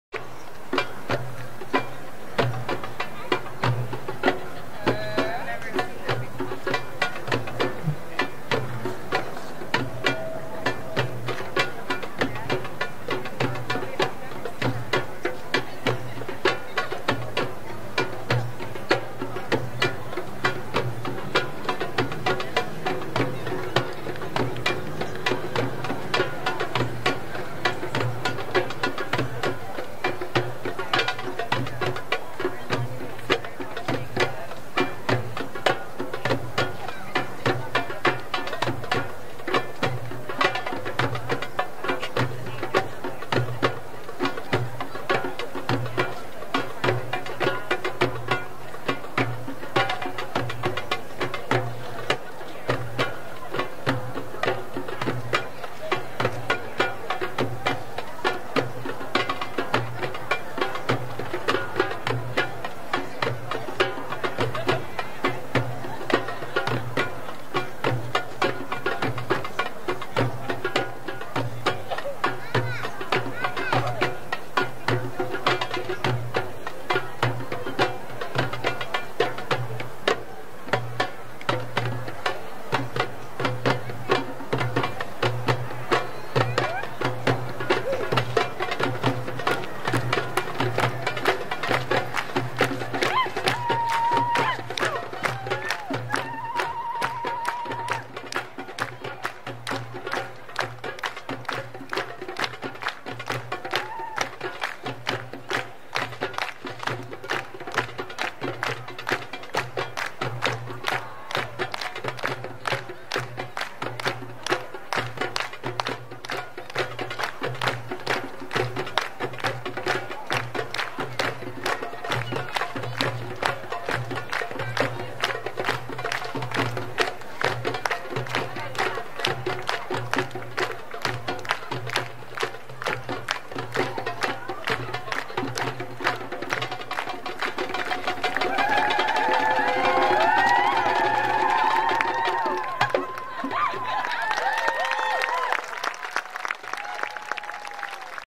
Live Drumming Tribal Fusion Belly Dance with Lindsey Feeney Dark Follies
July 2010 Dark Follies as part of First Friday Art Walk at Monument Square in Portland, Maine dancing with Steve Carpenter, Bragita Noreen, Brent Nelson, Nikki Shields, Bryan Burns.